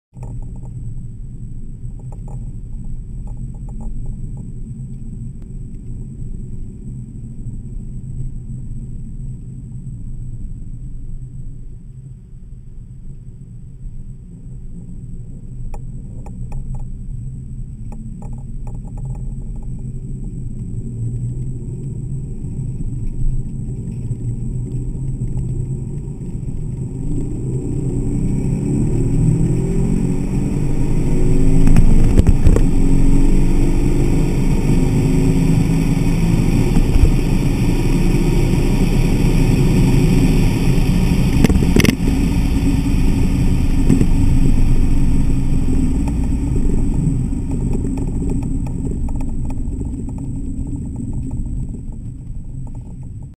60-130 Run in an RSK04 powered B5 S4
Run performed at Shift-S3ctor's airstrip attack, on a private runway.

2000 Audi S4
OEM RS4 K04 turbos
Custom GIAC switchable software for 52# injectors
Evolution Racewerks intercoolers
Southbend clutch
Bosch 044 intank fuel pump
upgraded cams
FSI coilpack conversion
2bennett 3" downpipes
3" exhaust